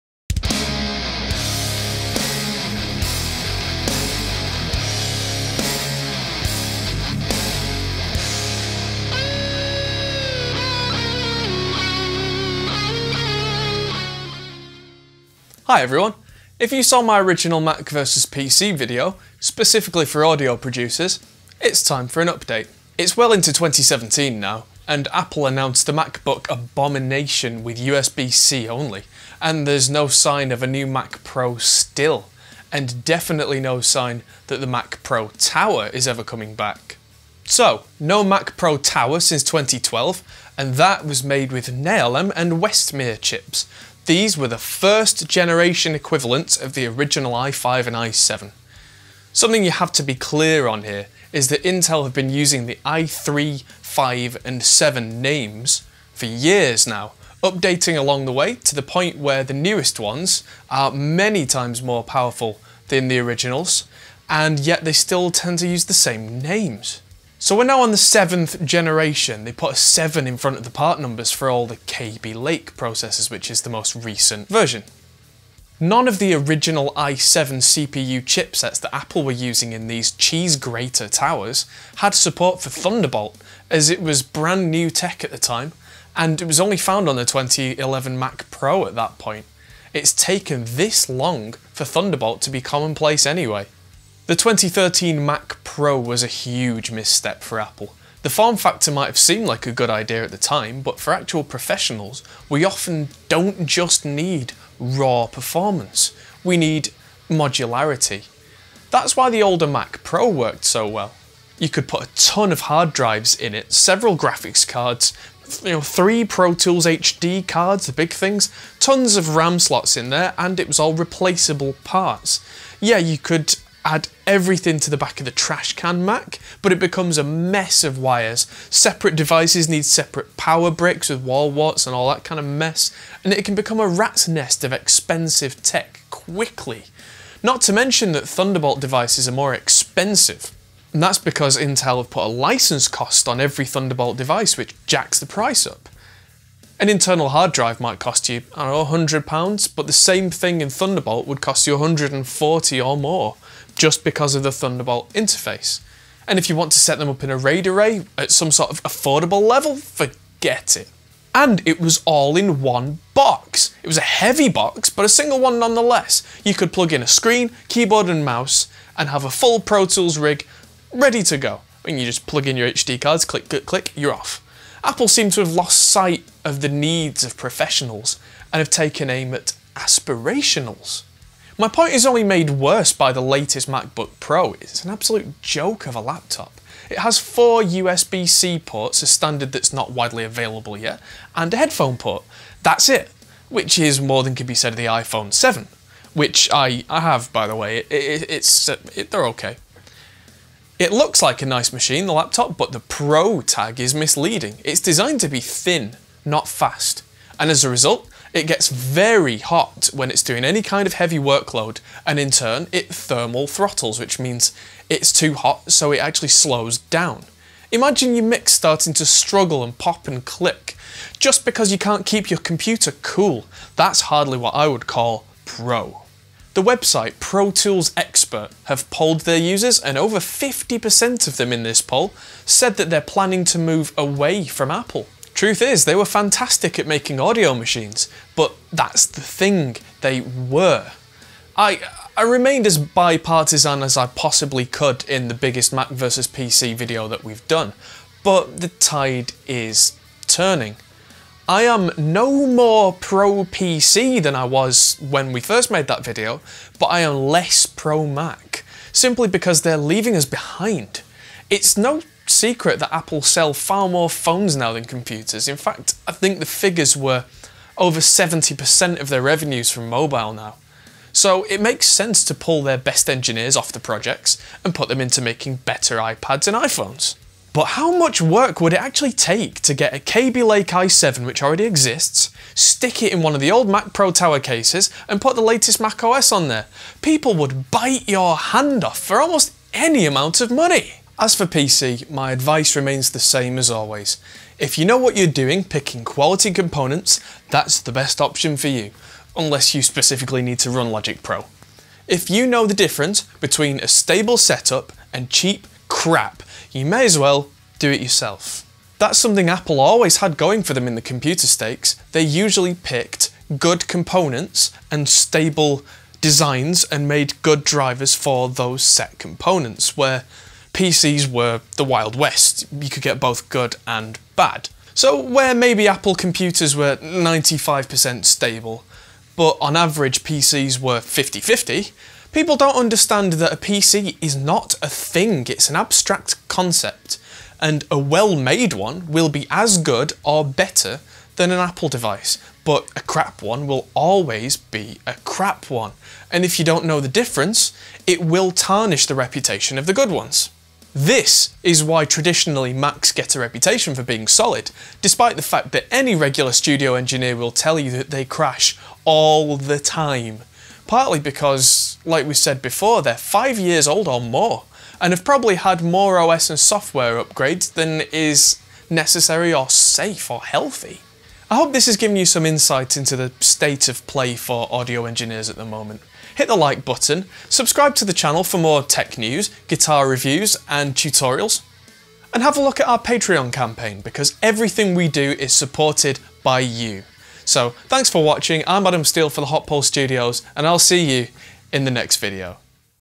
Mac Vs PC for Audio Production:- 2017 Update
Since the last Mac Vs PC video that we made, aimed squarely at music producers and in part video editors, a lot has happened in the Apple camp, and just as much hasn't happened.

There's still no sign of a new Mac Pro Tower, and even the trashcan Mac Pro hasn't seen an update in years. The new Macbook Pro has a touch bar and apparently performs WORSE for audio guys than the last generation, not to mention the lack of ports except for USB-C Thunderbolt, which is useful but not in isolation.

Thanks to our Patrons who made this possible - your names are at the end of the video. You make a huge difference!